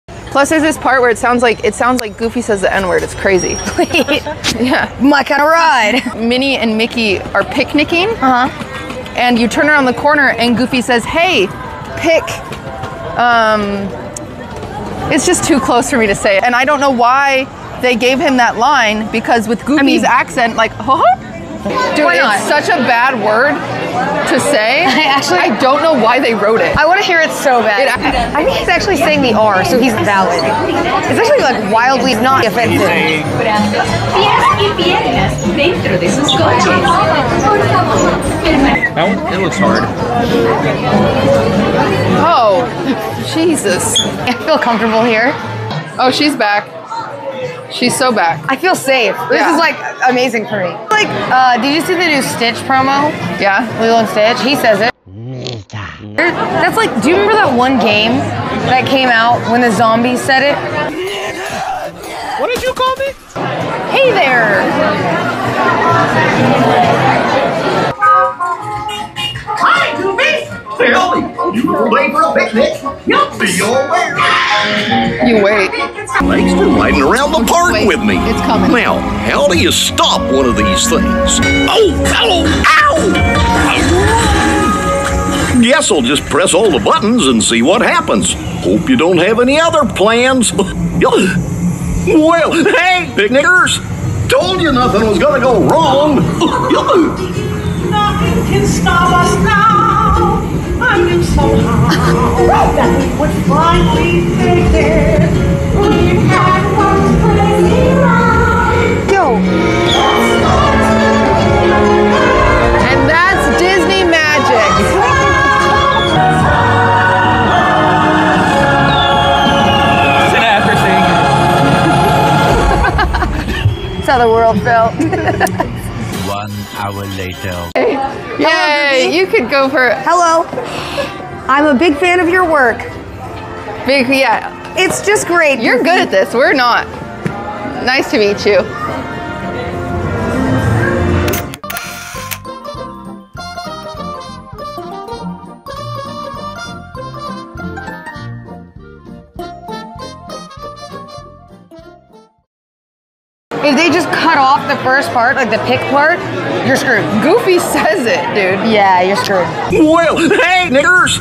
QT Shows Cinna The N Word At Disneyland
QTCinderella takes Cinna to Disneyland and finds her a new favorite ride! Goofy sure loves picnics! haha!